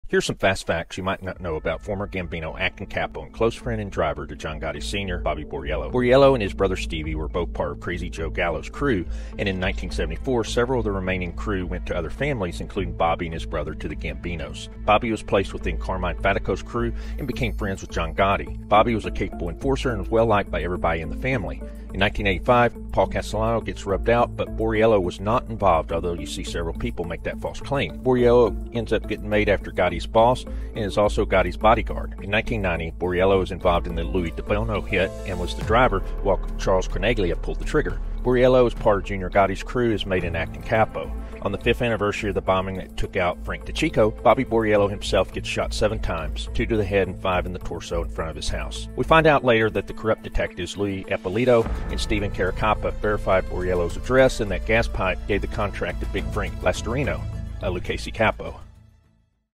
MOB RUBOUT THAT DEVASTATED JOHN GOTTI AND THE GAMBINO'S "BOBBY BORIELLO"
Bobby Boriello was the close friend and driver bodyguard of Gambino boss John Gotti.  He and his brother Stevie were part of Crazy Joe Gallo’s crew originally and in 1974 after the rubout at Umbertos, the crew was split up and members went to other families.  Bobby Boriello and his brother Stevie went to the Gambino family and was placed into Carmine “Charley Wagons” Fatico Bergin crew where Bobby became fast friends with John Gotti.  In 1985 John Gotti and @officialsammythebull Sammy Gravano along with others orchestrate the take over of the Gambino family and John Gotti is made boss.  Soon, Bobby Boriello is made the personal driver for John Gotti.  Boriello had a reputation for being a capable enforcer but was a well liked guy.  He also was a family man.  He had some issues with an associate of the Genovese family Preston Geritano and in fact Boriello reportedly tried to take him out.  In 1990, Boriello is put on the hit against Louie DiBono. Boriello is the driver and he along with McMahon and Charles Carneglia find Louie DiBono down in the parking lot of the World Trade Center.  charles Carneglia is the one who does the deed.  Shortly after this rubout, Bobby Boriello is promoted to an Acting Capo position and is working for John Gotti Junior.  Chin Gigante and Gaspipe Casso are still plotting to take out Gotti after the Paul Castellano rubout in front of Sparks which was done without approval.  We know that in April 13, 1986 Underboss to the Family Frank Decicco is taken out by a device in the car.  The thought was that the zips might have done it but it was orchestrated by the Lucchese Underboss Anthony Gaspipe Casso.  We also know they take care of Eddie Lino as well.  So, this day exactly 5 years later April 13, 1991 Bobby Boriello had just left the Bergin Hunt and Fish Club where he had a lunch with the gambinos.  Unfortunately for Boriello, the two crooked detectives on payroll Louie Eppolito and Stephen Caracappa had visited Boriellos wife a few days earlier and confirmed that Bobby lived in this Bensonhurst address.  Gaspipe gives the contract to Lucchese Capo Big Frank Lastorino who pops 7 into Boriello. 2 to the head and 5 in the torso.  Boriello is instantly no longer among the living.  His wife was home and heard the commotion and goes out and sees her husband on the side of the car and sleeping with the fishes.  John Gotti was devastated and so was most of the family but no retaliation takes place because the family had its own problems to deal with from a legal standpoint. Initially it was thought that it was Preston Geritano who did the act because of the past beef but that was not the case.  It was all part of Chin Gigante and Gaspipe Casso to put the noose around the upper regions of John Gotti and eventually target him.  Boriello was a very important member of the Gambino family.